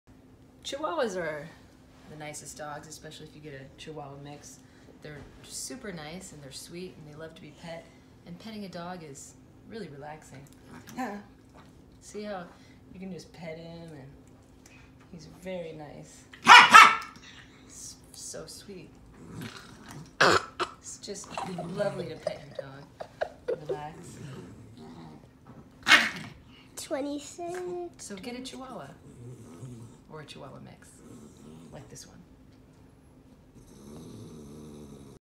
Why chihuahuas are the best
Chihuahua dog mix named Ivan who loves to be pet